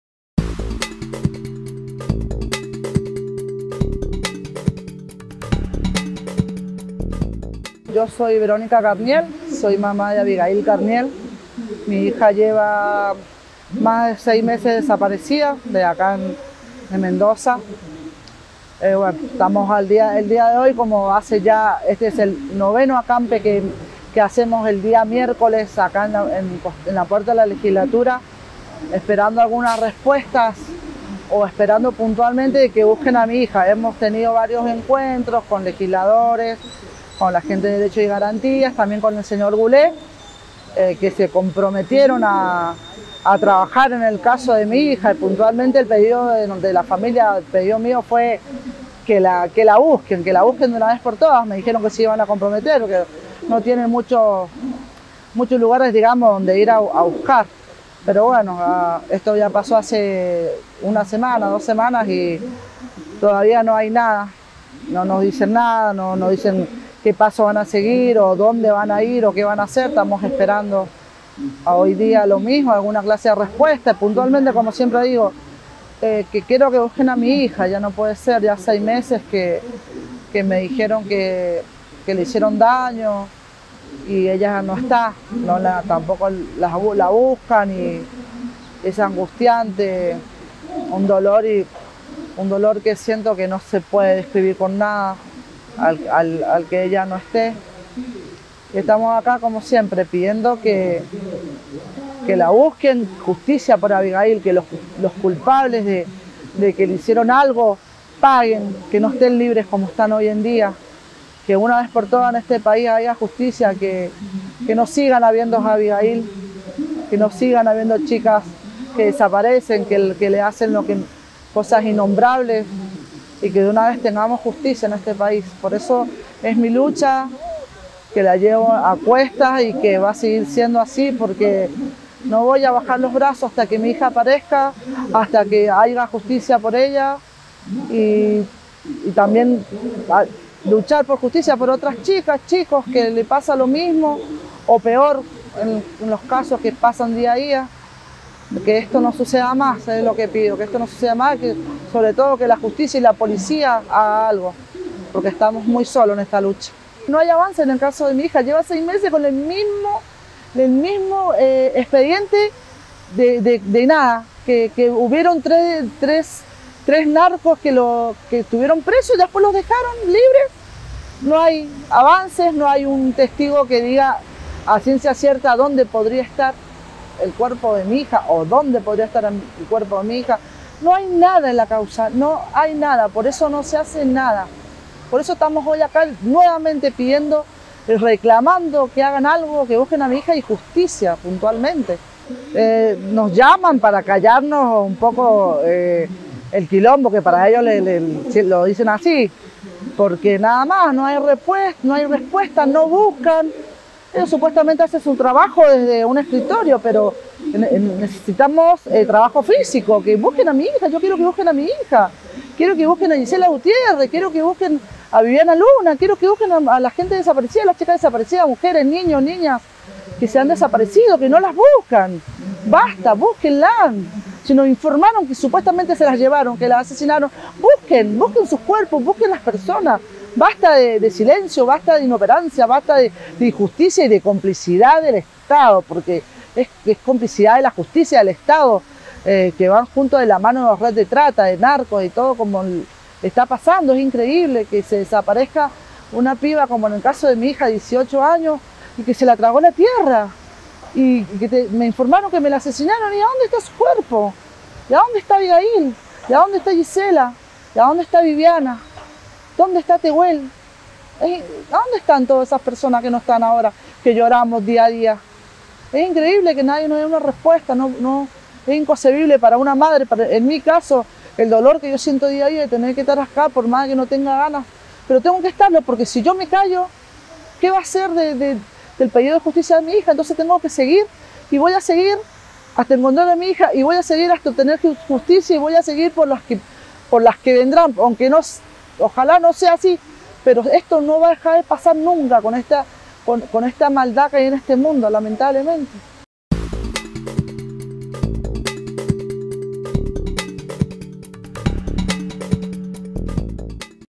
9° acampe por Abigail Carniel
A seis meses de la desaparición de Abigail, su madre, familia, amigos, amigas y organizaciones sociales continúan exigiendo que se investigue y haya avance reales en la causa. Sin embargo, no hay información acerca de qué pasó y dónde esta Abigail, pese a que hace dos semanas se concreto una reunión con el procurador Alejandro Gullé y el fiscal Fernando Guzzo donde se comprometieron a avanzar de manera urgente en la causa.
Escuchamos la entrevista a Verónica Carniel, madre de Abigail quien todos los miércoles realiza un acampe y marcha desde la legislatura provincial hasta el poder judicial.